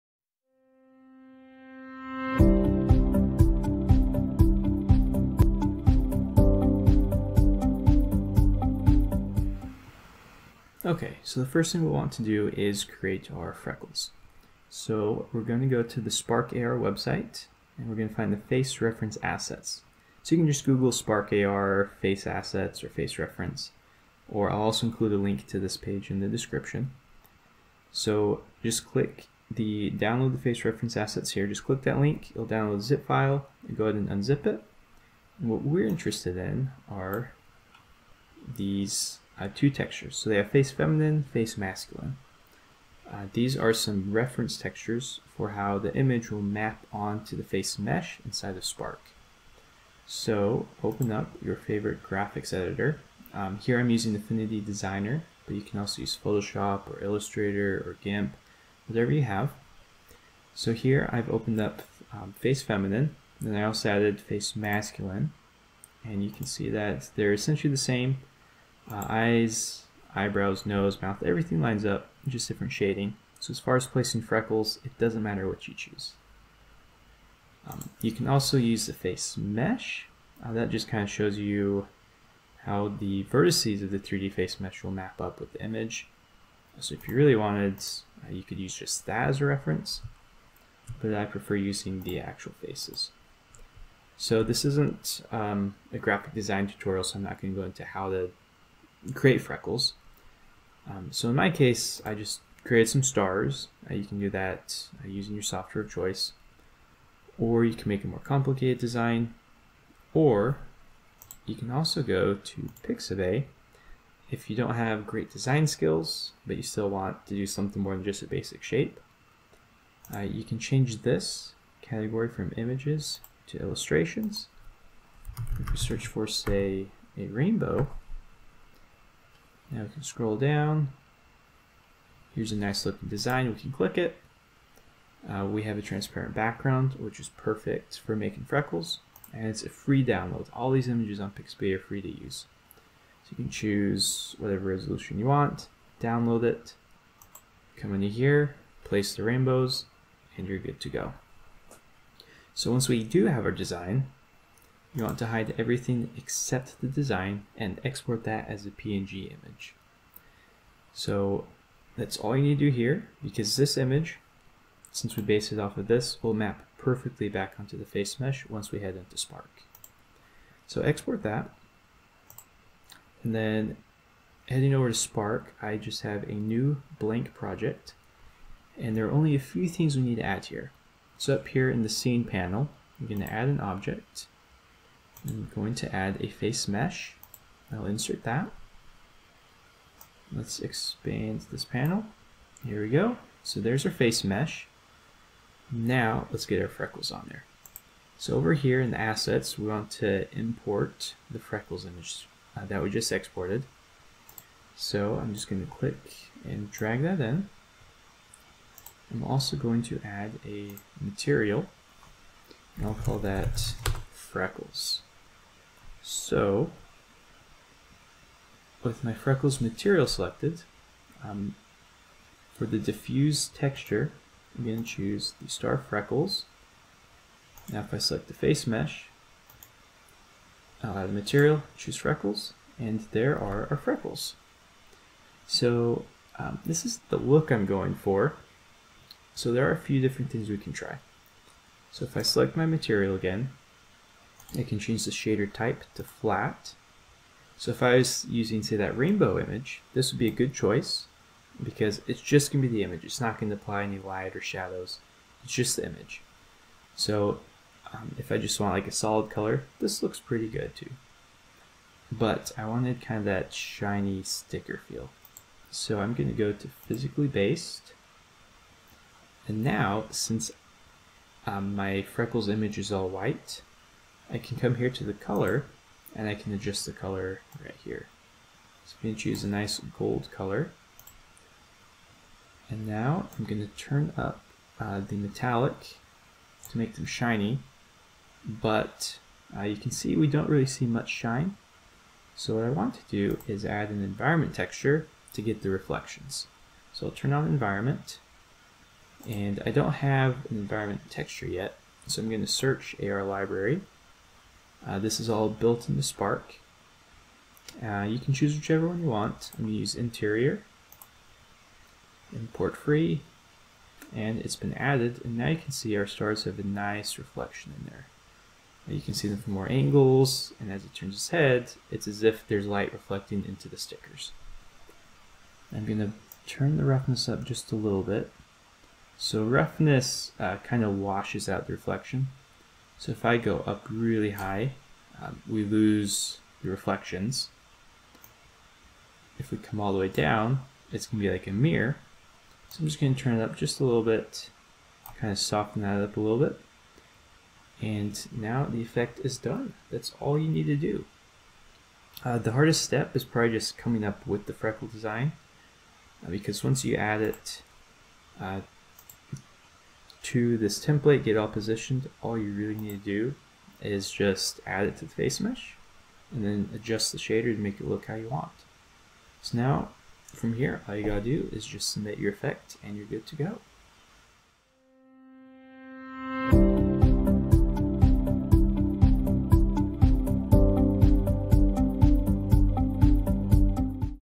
Easy Freckles in Spark AR!
Learn how to easily make freckles in Spark AR Studio! You'll learn how to use the Face Reference assets as a template for placing the freckles. Then all you need to do is add a face mesh, add a new material, and you're ready to roll!

Here is the link to download the Spark AR Face Reference Assets

Like what you see? In addition to the tutorials, I also teach people about bugs. It's fun!